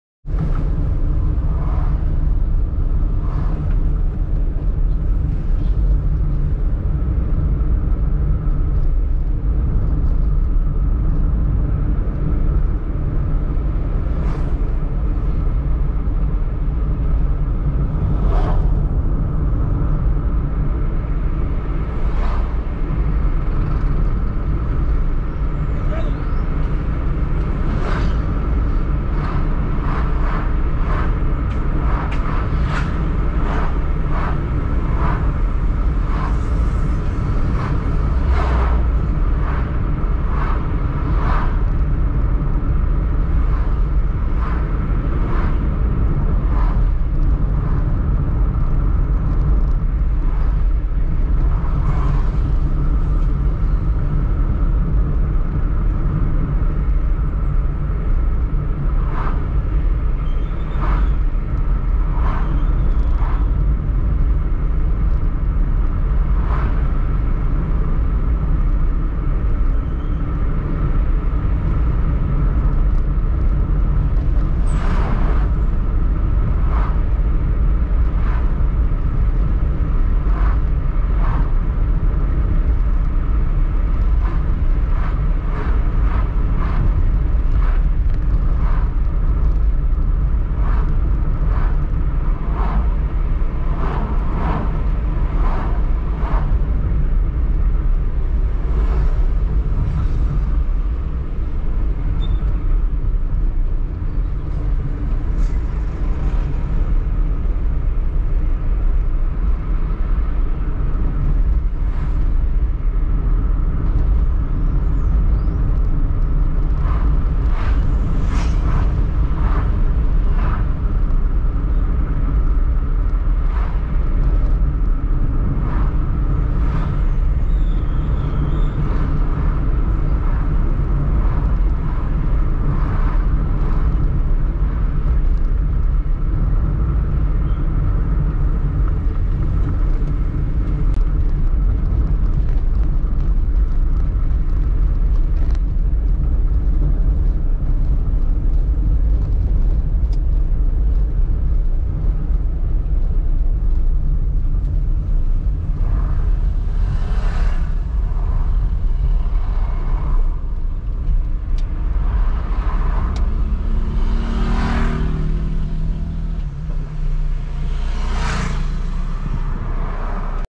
AVENIDA PROTÁSIO ALVES, PORTO ALEGRE. 4K
IMAGENS DA AVENIDA, QUASE NA CIDADE DE VIAMÃO.
BAIRRO MÁRIO QUINTANA.